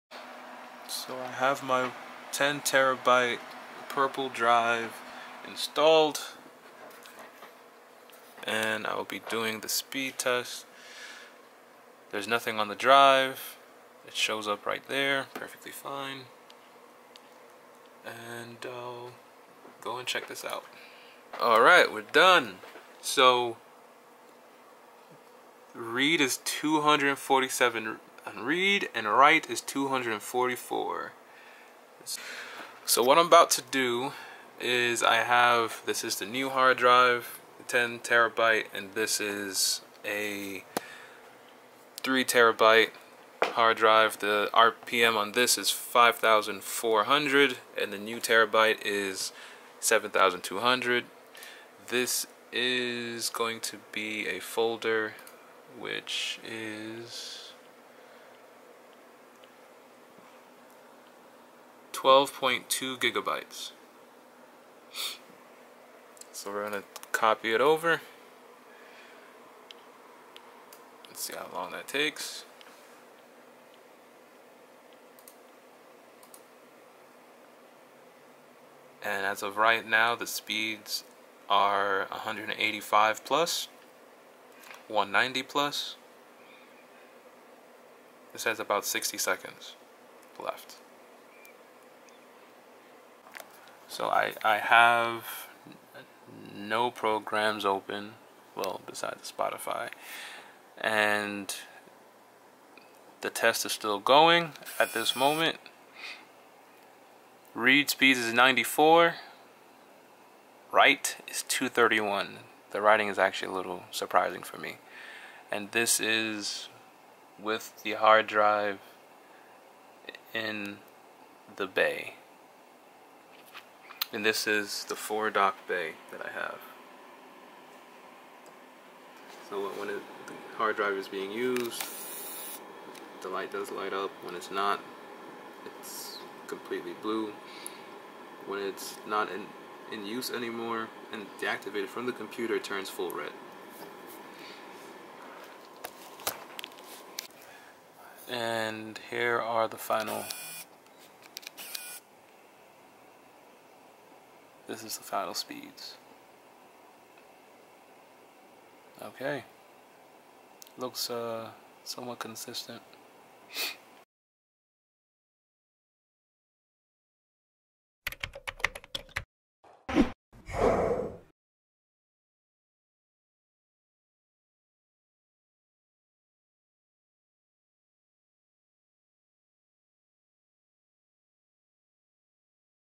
Sabrent 4 bay docking station Speed Test
This is a follow-up video to show the speed of the Sabrent 4 Bay dock compared to the conventional method of plugging the Hard drive straight into the computer via a SATA cable. The Sabrent 4 bay dock is connected to a 3.2 USB C port.